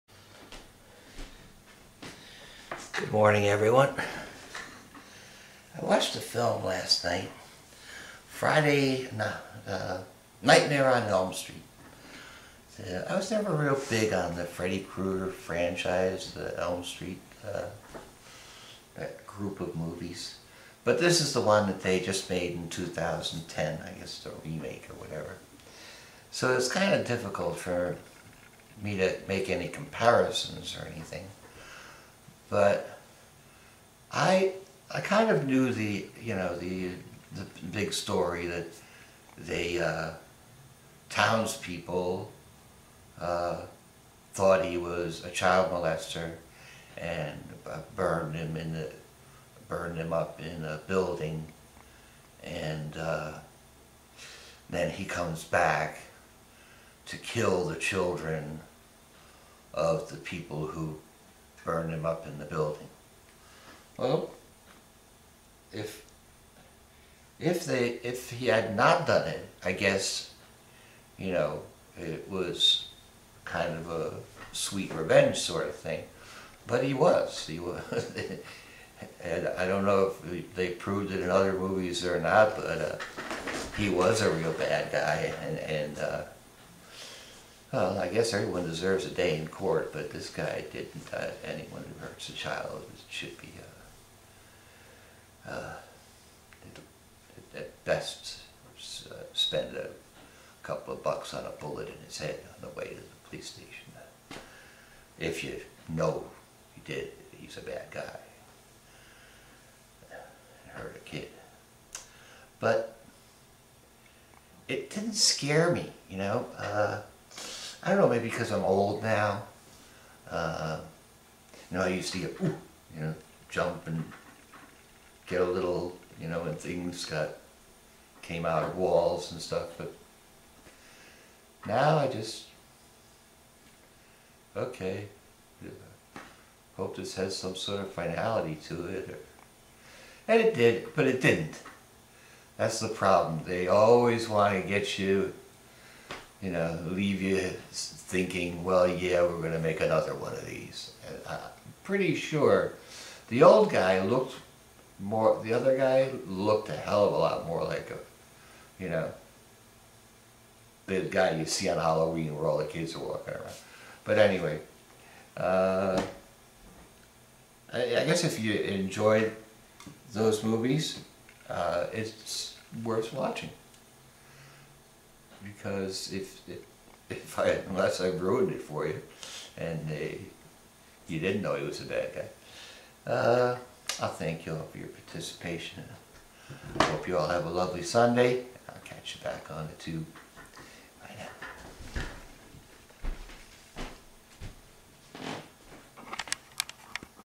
Nightmare on Elm Street 2010
My opinion on the remake of Nightmare on Elm Street